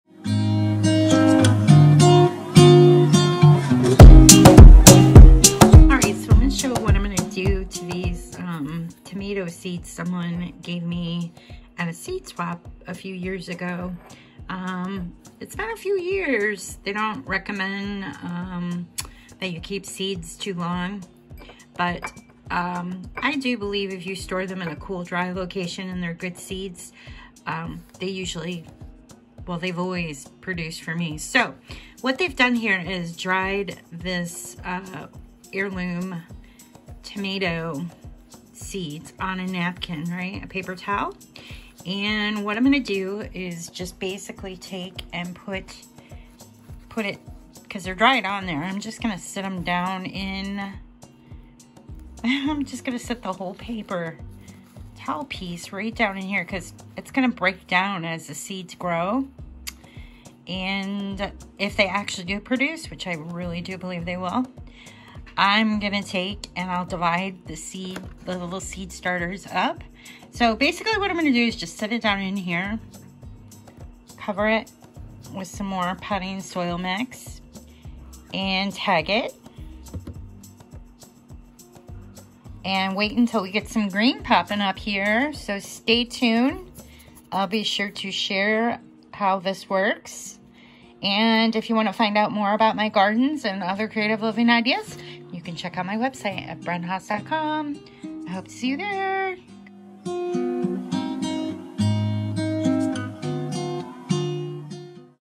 Seed starting from Paper Towel
In this short video I share how I am planting a piece of paper towel that has a few tomato seeds dried onto it.